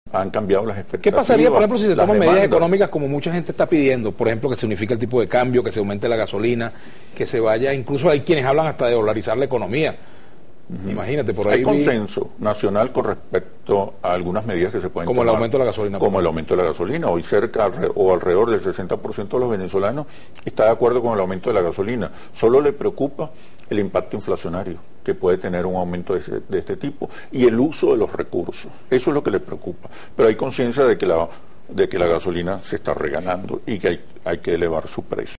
Schemel: El 60% de los venezolanos está de acuerdo con el aumento de la gasolina
Sobre las medidas económicas a ser tomadas en el país, el director de Hinterlaces, Oscar Schemel, indicó, este lunes, que el incremento de la gasolina es una de ellas. “El 60% de los venezolanos está de acuerdo, sabe que la gasolina se está regalando”, dijo.